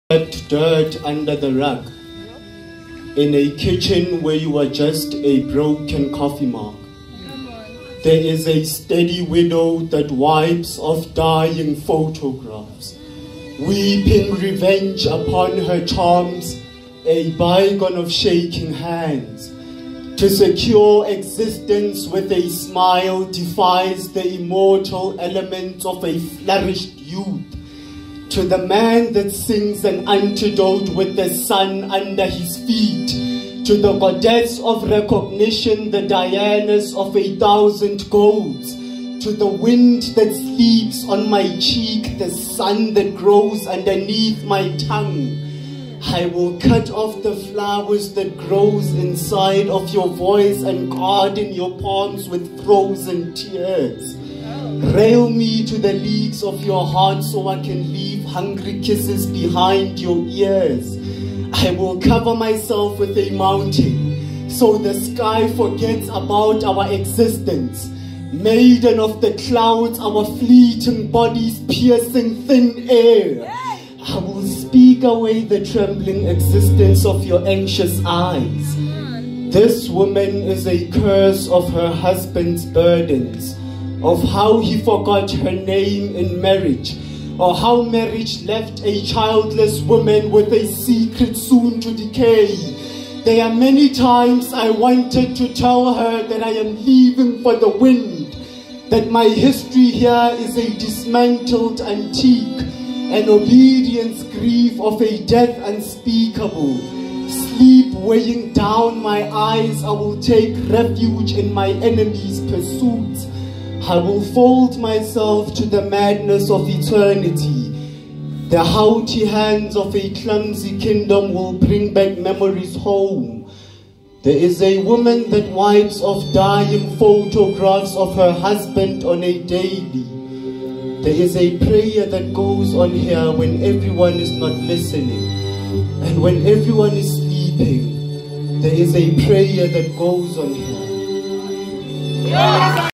Lunga _ S8Ep6
Lunga performs at the Word N Sound Poetry League, Season 8 Episode 6.

July 2018
Market Theatre Lab, Newtown Johannesburg